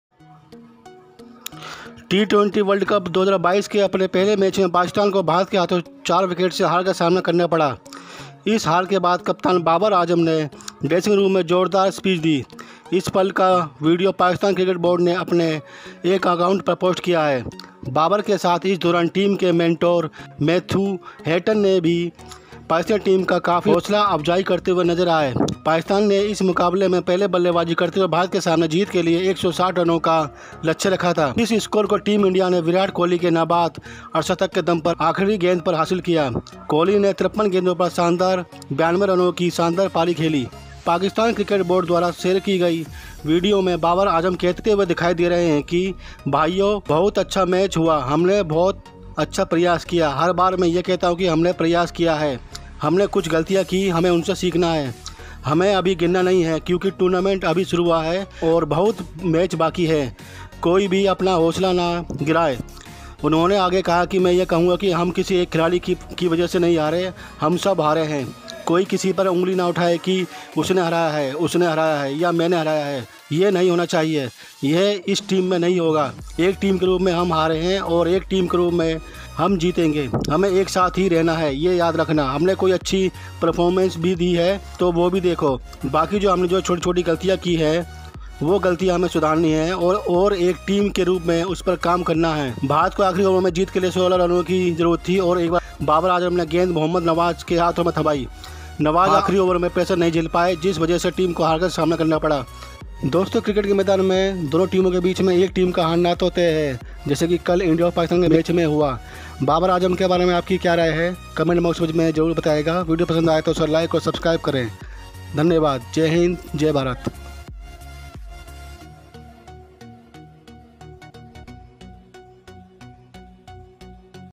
babar azam interview | हार के बाद बोले बाबर आजम
Hello friends Cricket Bolt Channel mai aap sabhi ka hardik Swagat hai. Doston is Channel mai aapko Cricket se related updates milti rahegi.

Hello friends India aur Pakistan ke bich hue match mein Pakistan ko Mili Haar ke bad Babar Azam bole Ki humne abhi tak himmat nahin Hari hai ham agale match mein achcha match khelenge ham Jeet ke dikhayenge doston Aaj ke video mein yah sab dikhaya hai video ko complete dekhen video Pasand aaya to like aur subscribe Karen
Thanks